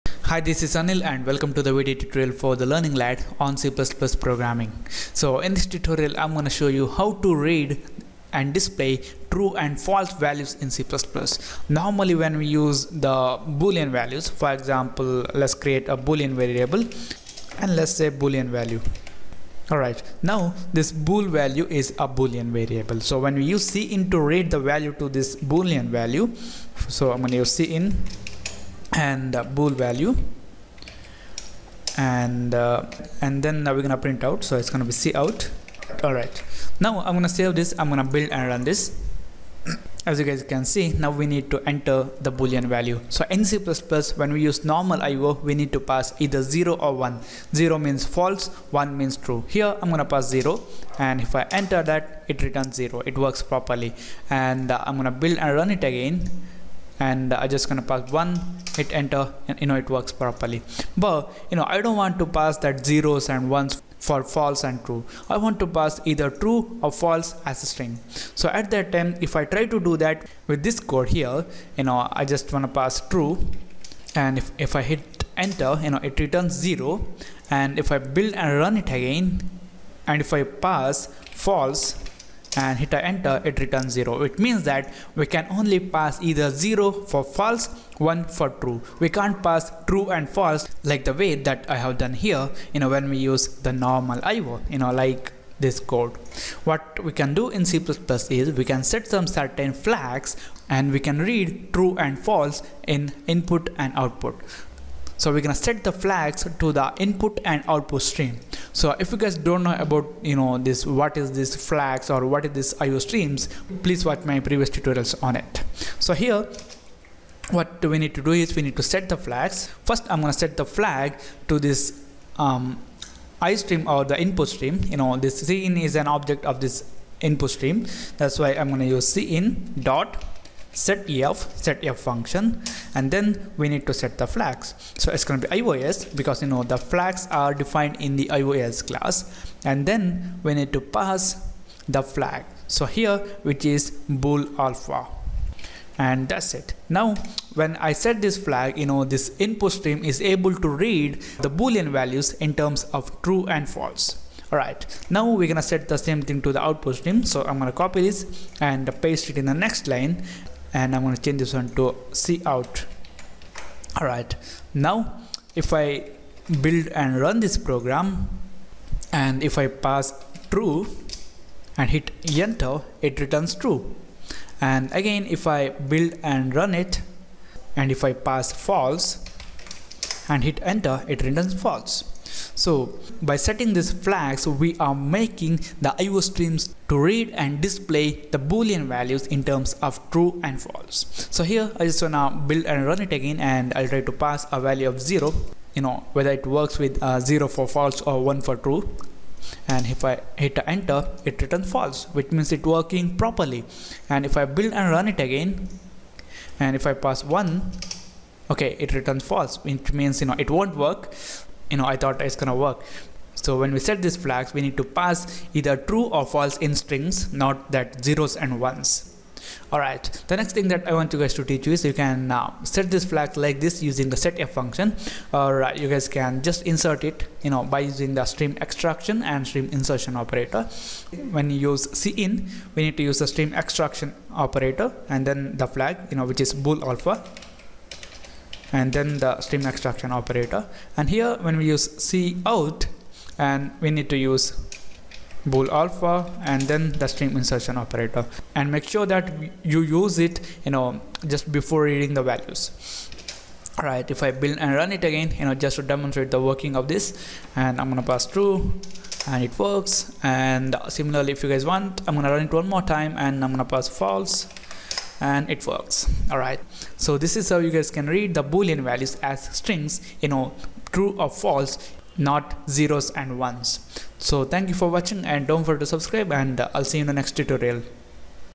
Reading and Displaying Boolean Values as TRUE and FALSE instead of 0 and 1
In this c++ Video tutorial, you will learn how to read and display Boolean values as true and false instead of  1 and 0.

You are gonna learn  how to set/use boolalpha flag to make the io to interpret  Boolean values as true and false in detail with example.